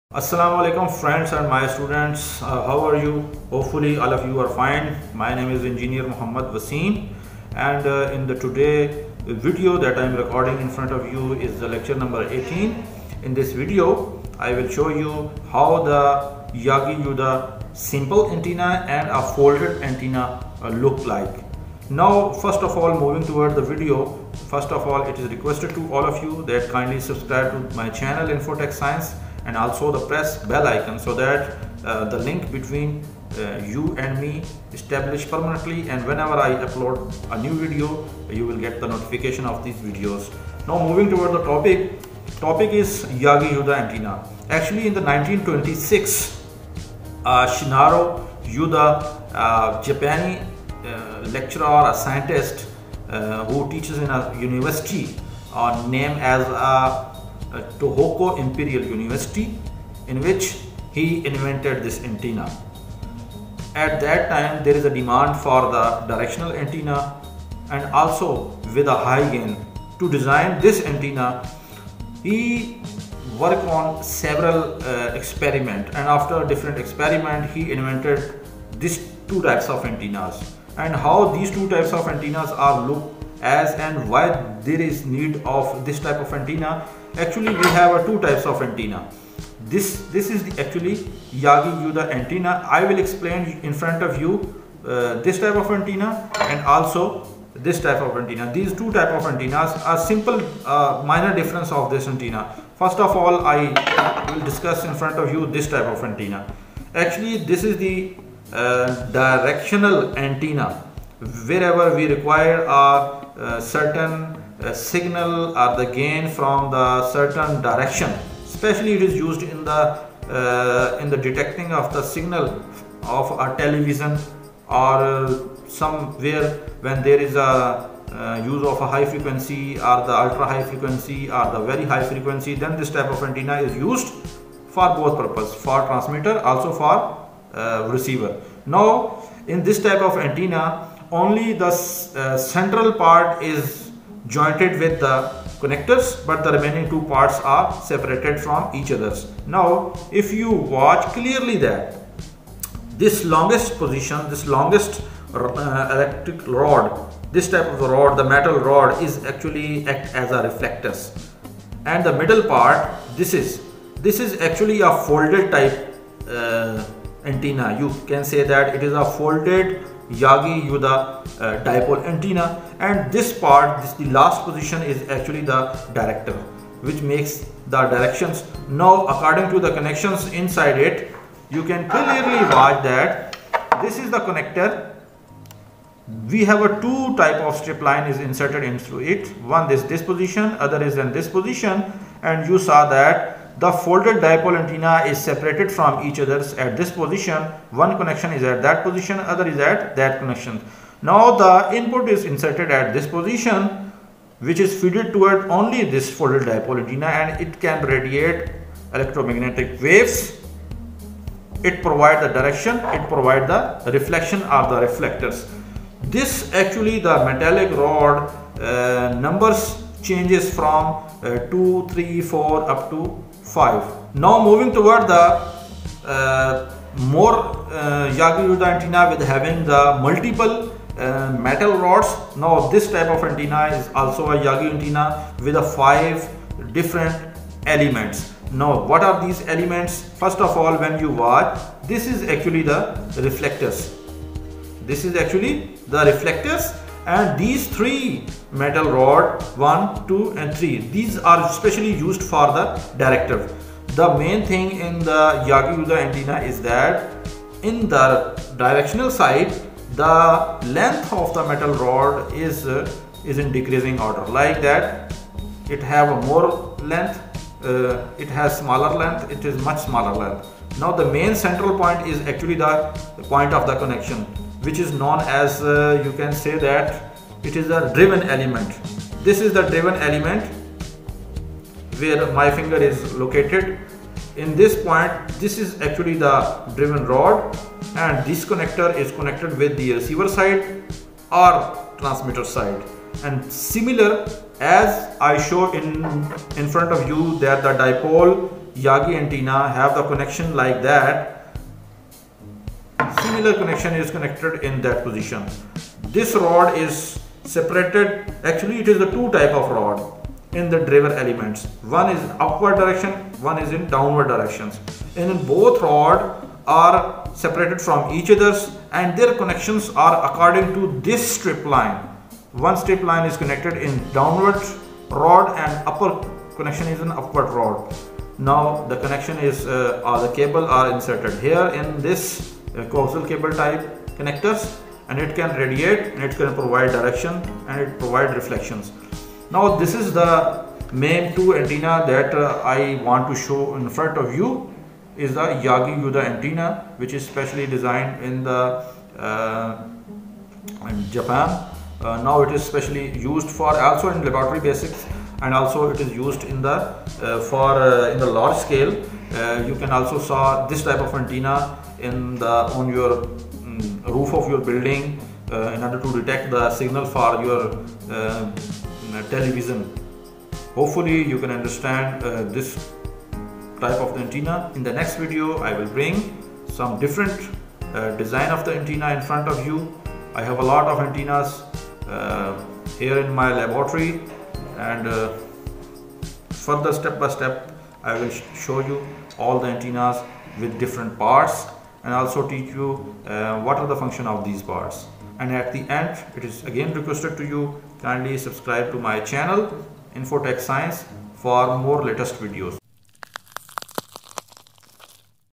What is Yagi antenna| How to design Yagi Uda folded dipole antenna
This video is related to an directional antenna Yagi-Uda antenna how to design this antenna what are the parts of this antenna, what is reflector, what is directive antenna